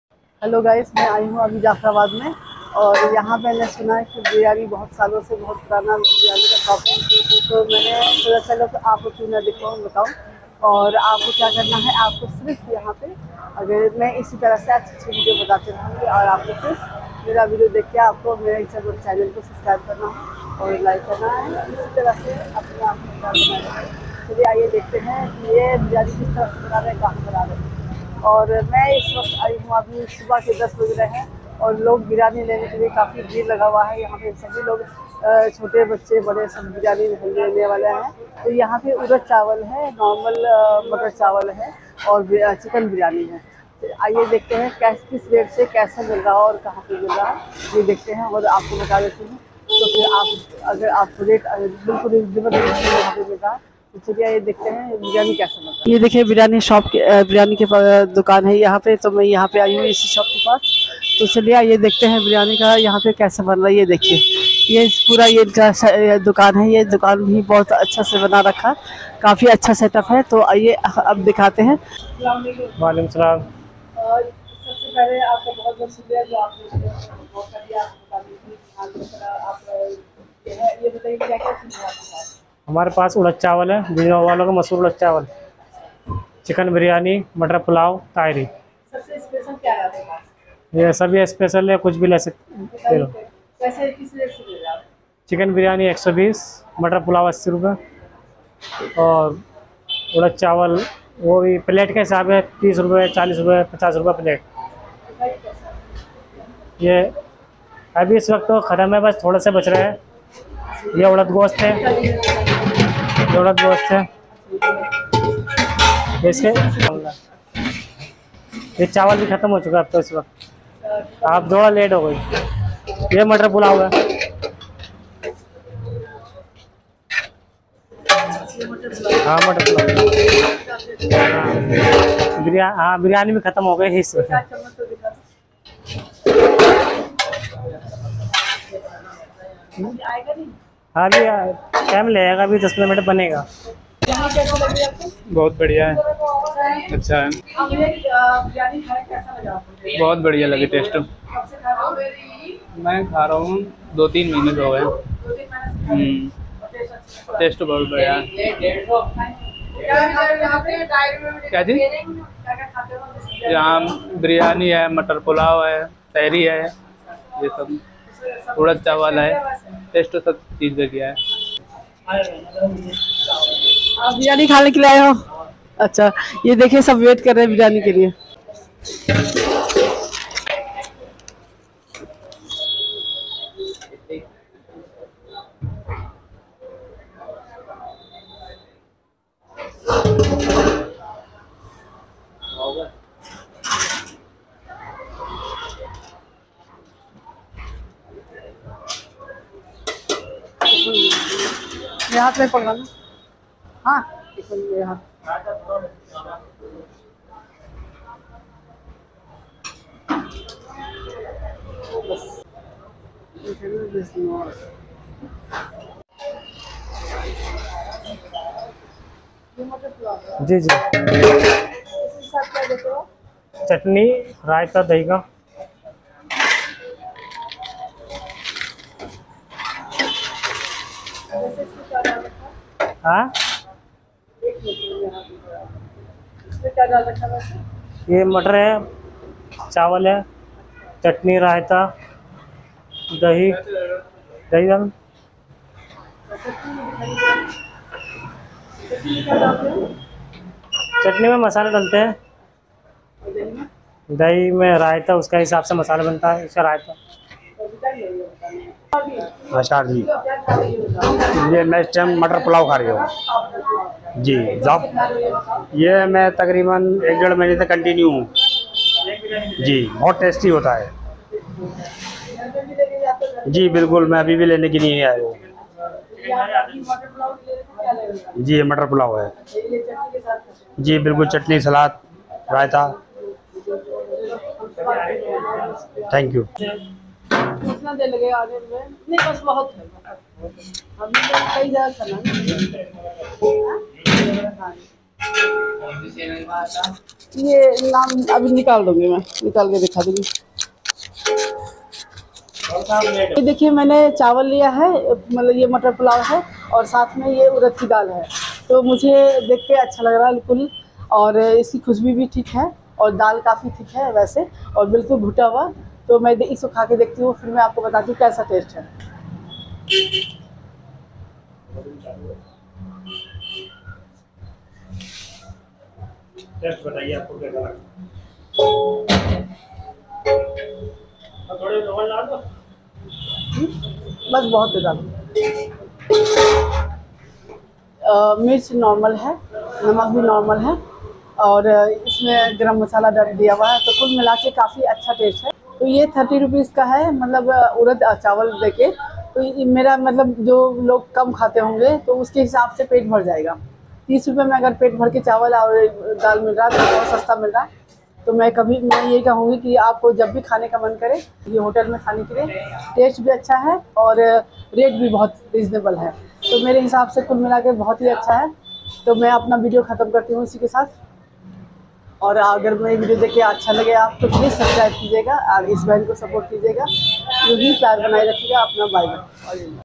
Delhi ka famous chicken biryani matar chawal or udad ki dal Delhi food street food
jafrabad ka famous chicken biryani urad chawal aur matar chawal Delhi food street food